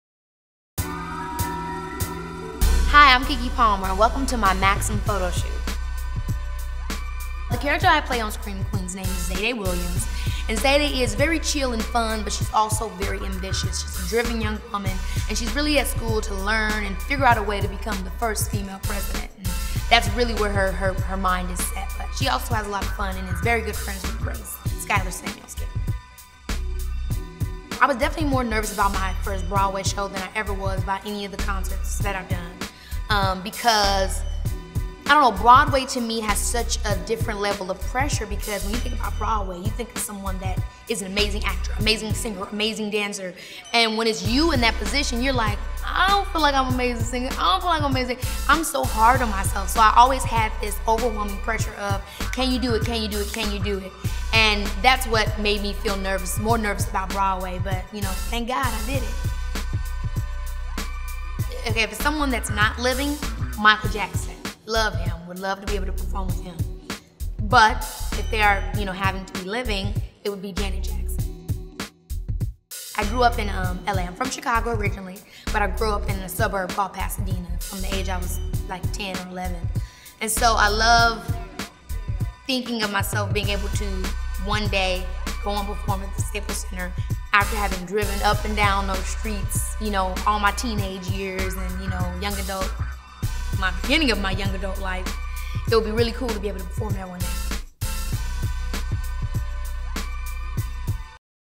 Keke Palmer: American Scream Queen
Keke Palmer answers Maxim's most pressing questions.